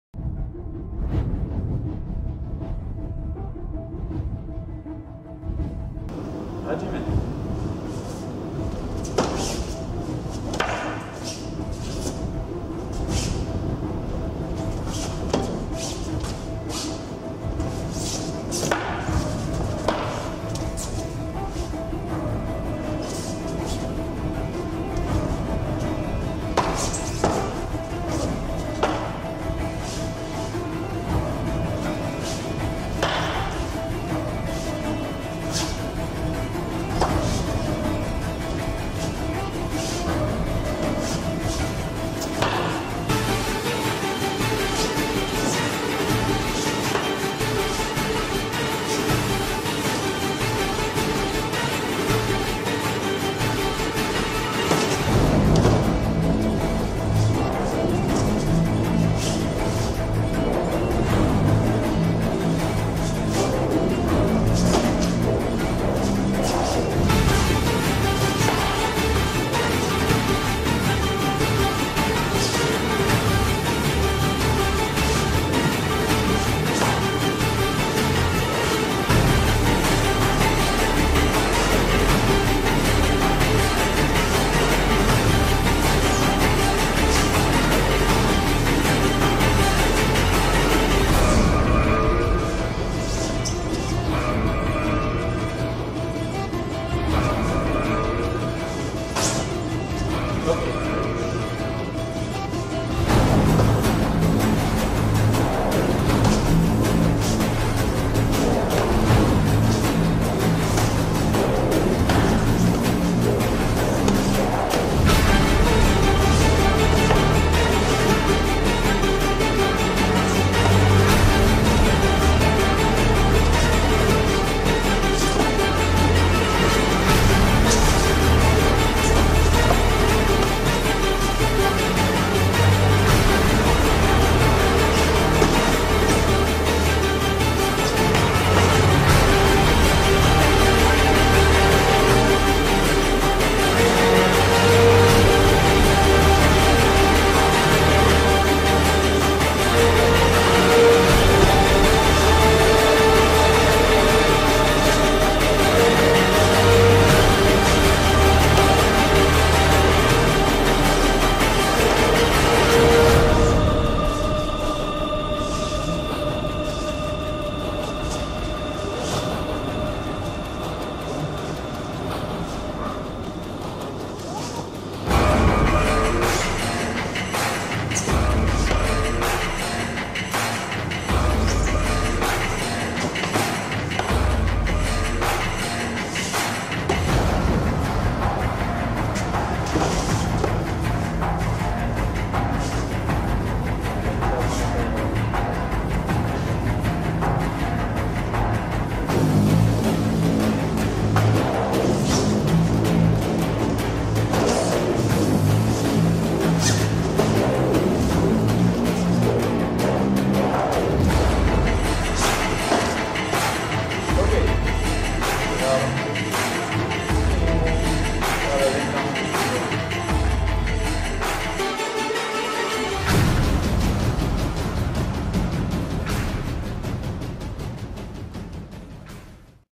Yari Katana Randori - Bujinkan san-Dan test
Yari (Spear) and Katana (sword) Randori
Bujinkan Ninjutsu Israel
A short part form the San-Dan test of Shay Aharon and Zohar Alon
Bujinkan